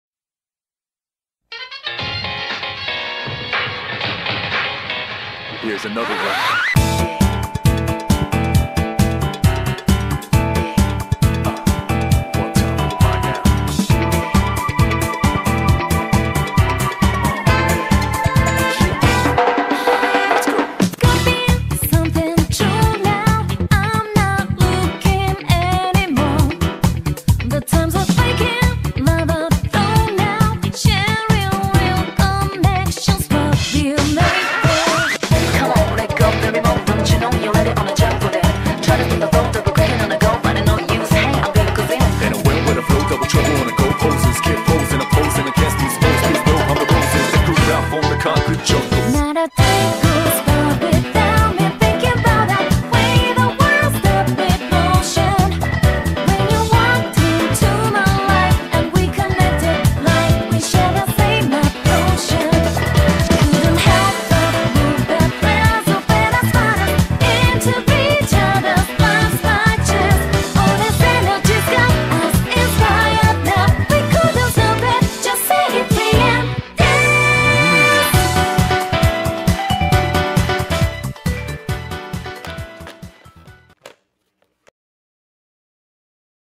[MMD] Miya in: dance!
Ed ecco un video MMD nel quale ritorna un'altro mio personaggino: Miya
Ha forse qualche difetto.. ma cercherò di sistemarla. Intanto fatemi sapere nei commenti che ne pensate.